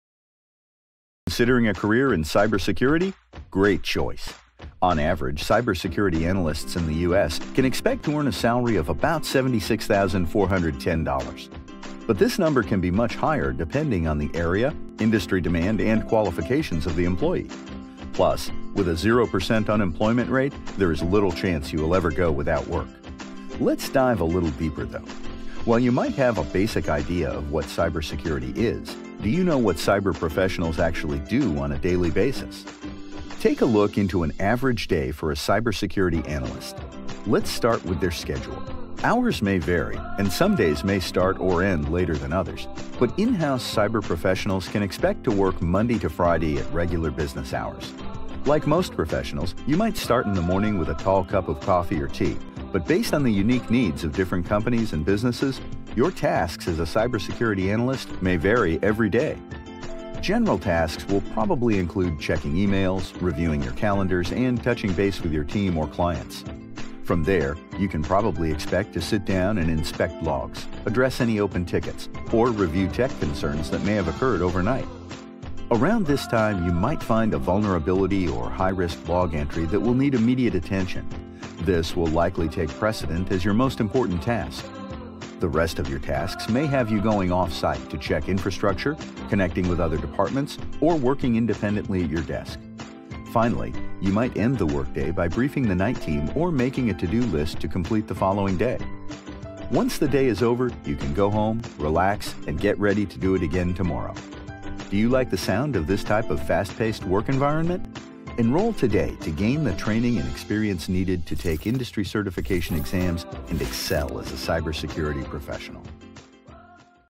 Discover a Day in the Life of a Cybersecurity Specialist
Go through a typical day with a cybersecurity professional and learn the skills often required to succeed in the IT and cybersecurity field.